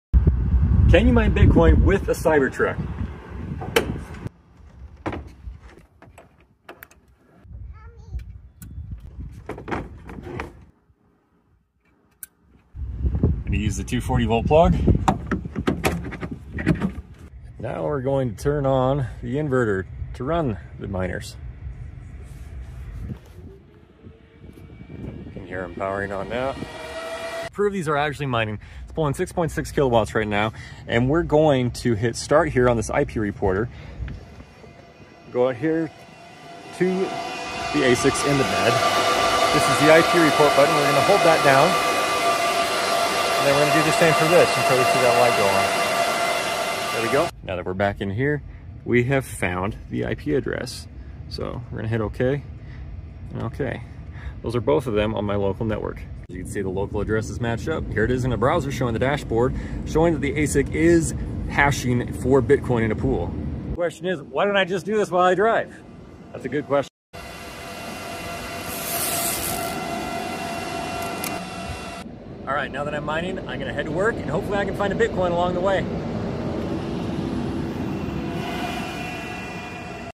🚨 A tutorial on how to mine Bitcoin in the back of a Cybertruck
Ever dreamed of mining Bitcoin on the go, powered by the futuristic Cybertruck?  In this (slightly tongue-in-cheek) tutorial, we explore the possibility of Bitcoin mining in the back of a Cybertruck.  We'll cover the (theoretical) power requirements, hardware considerations, and potential challenges (and maybe some fun misconceptions!) involved in such a project.  While it might not be practical (or even possible) for everyone, it's a fun thought experiment exploring the intersection of crypto, electric vehicles, and cutting-edge technology.  Join us as we break down the (mostly hypothetical) steps and discuss the future of mobile Bitcoin mining.